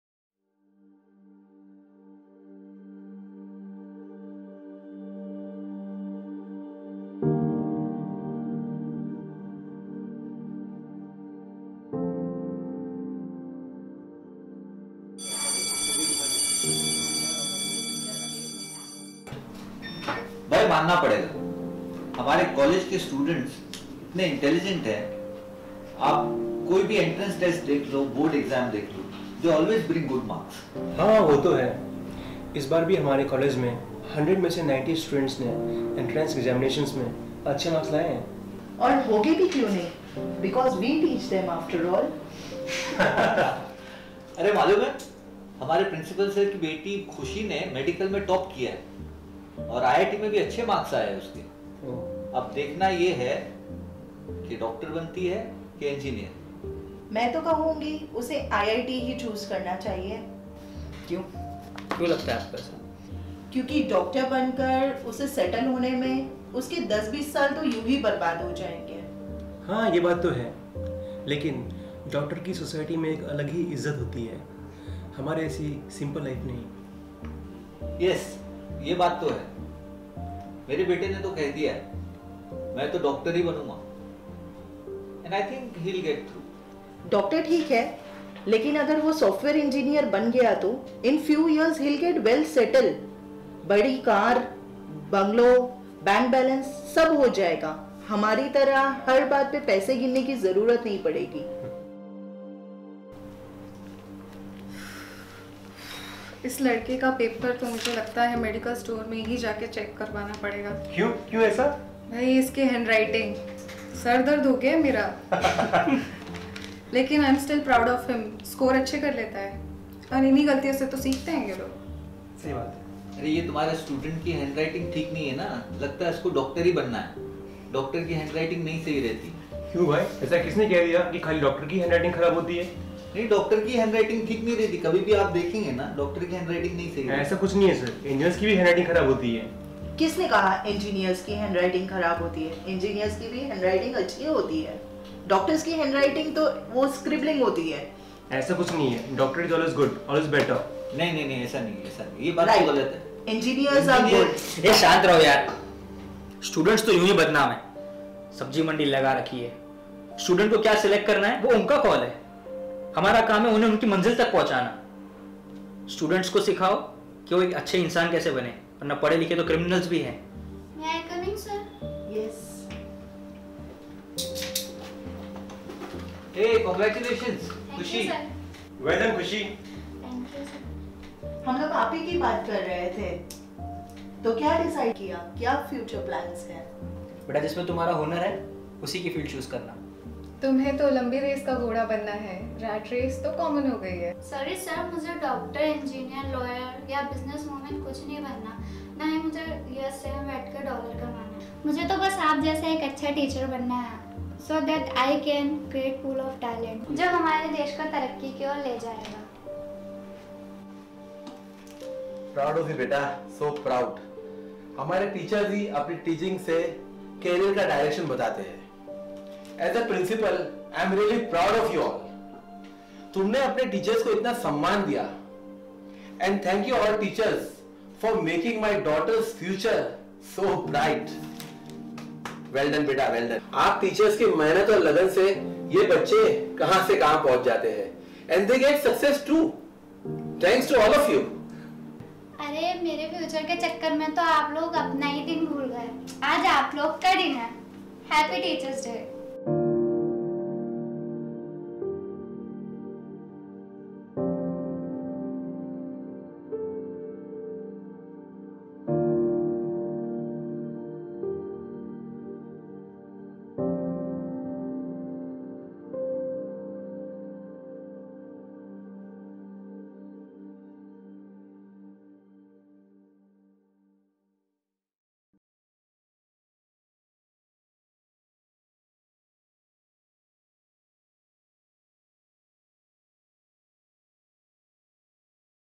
"STAFF ROOM" A Short film on *TEACHER'S DAY*
A Short film by CREATIVE BUDDIES.
we appreciate all the teacher's who teach and share their knowledge to the students and make them successful in life.